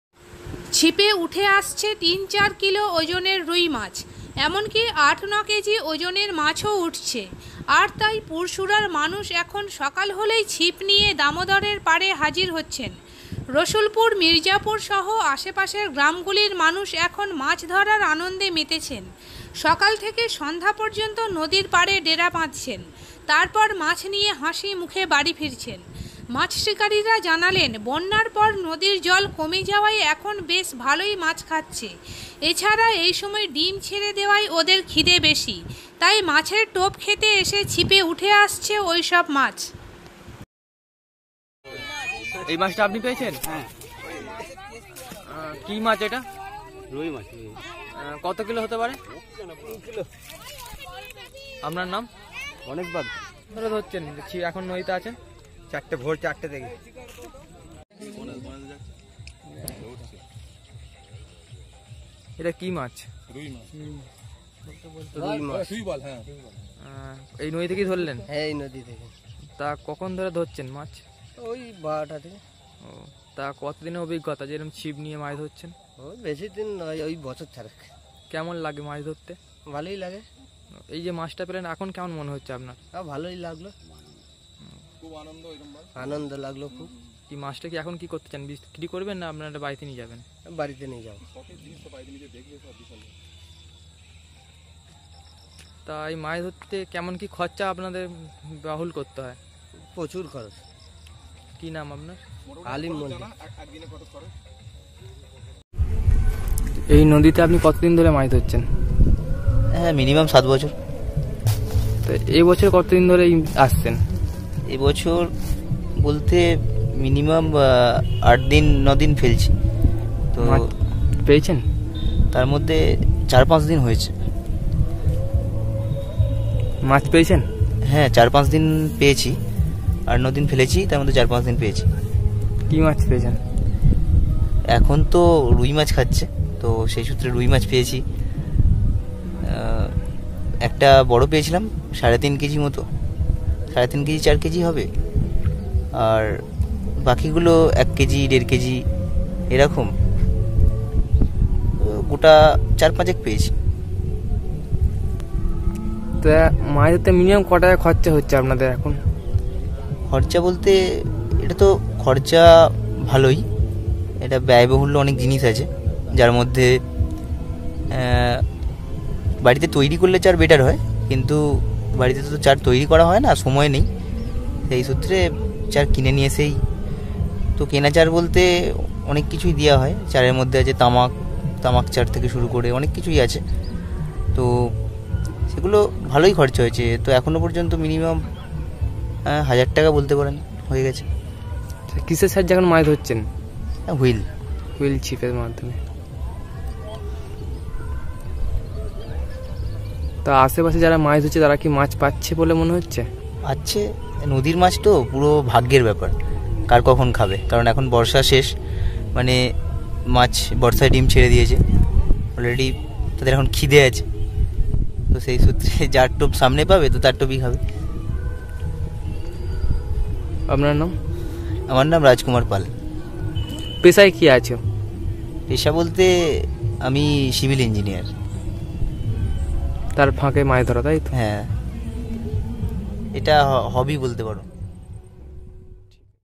পুড়শুড়ার দামোদরে ছিপে উঠে আসছে ৮-১০ কেজি ওজনের রুই মাছ
Sambad Somalia @ পুড়শুড়ার দামোদরে ছিপে উঠে আসছে ৮-১০ কেজি ওজনের রুই মাছ